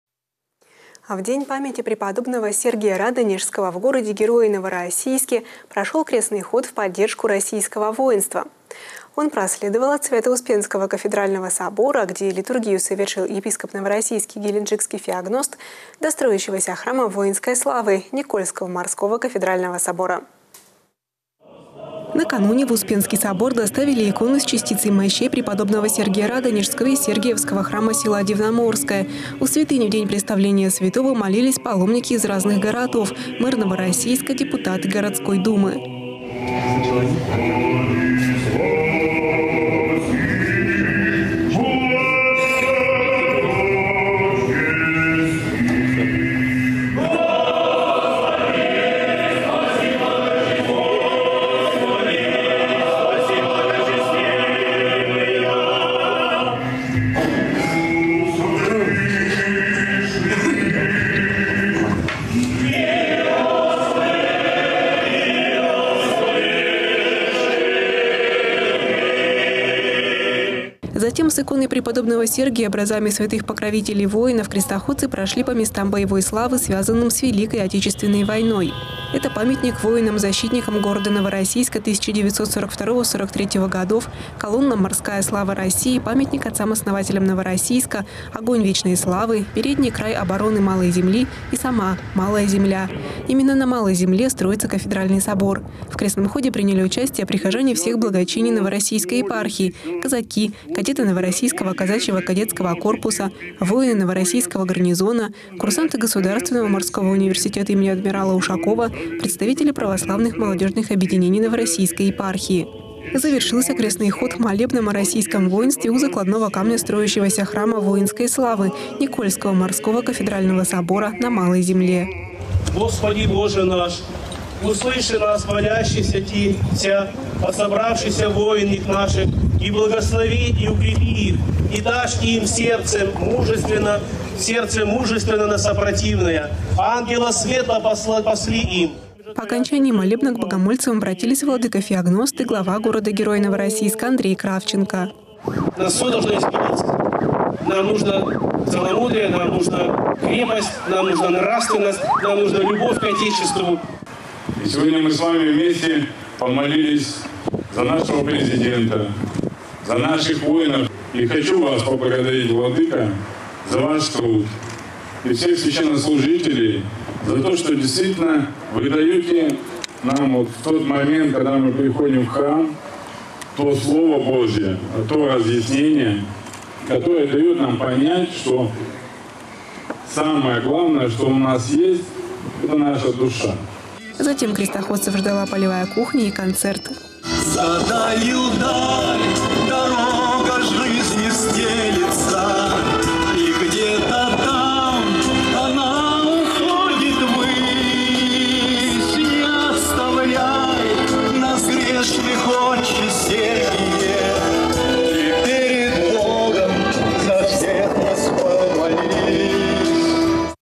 В городе-герое Новороссийске прошел крестный ход в поддержку российского воинства.
В день памяти преподобного Сергия Радонежского в городе-герое Новороссийске прошел крестный ход в поддержку российского воинства. Он проследовал от Свято-Успенского кафедрального собора, где литургию совершил епископ Новороссийский и Геленджикский Феогност, до строящегося храма воинской славы – Никольского морского кафедрального собора.